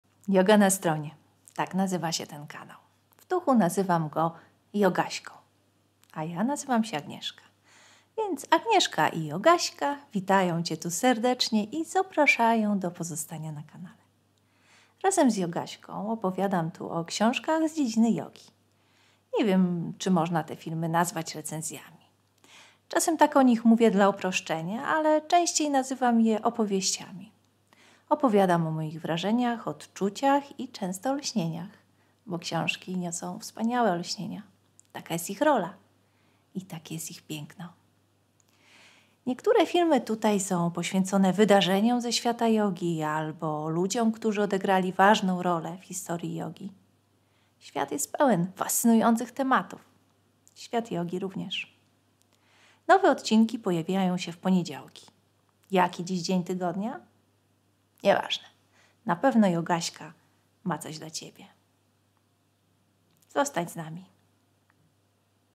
Powitanie na kanale Joga na stronie
Witam Cię na moim kanale Joga na stronie. Opowiadam tutaj o książkach z dziedziny jogi, mówię też o wydarzeniach ze świata jogi i ludziach, którzy odegrali rolę w jej historii. Mam nadzieję, że ze mną zostaniesz.

A na Spotify znajdziesz moje podcasty.